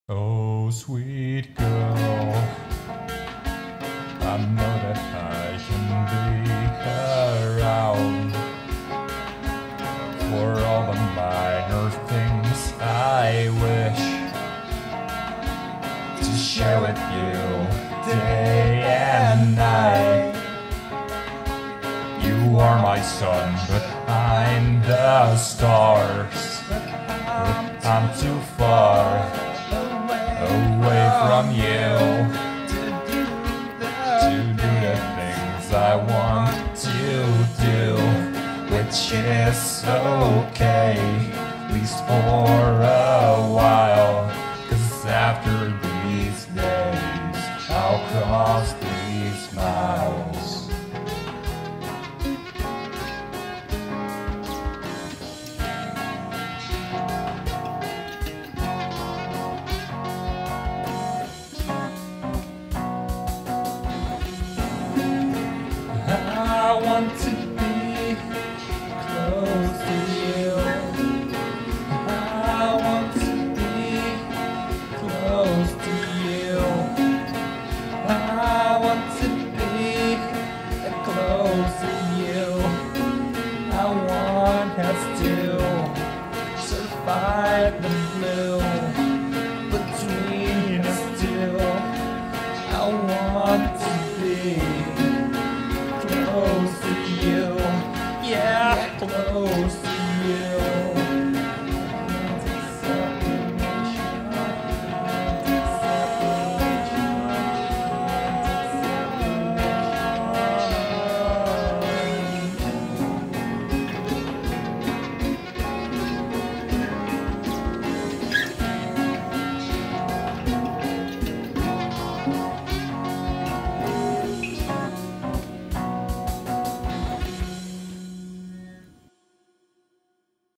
A short chaotic song I wrote to cope with the girl I love being offline rn
Not my best song or most profound song but it helped pass the time.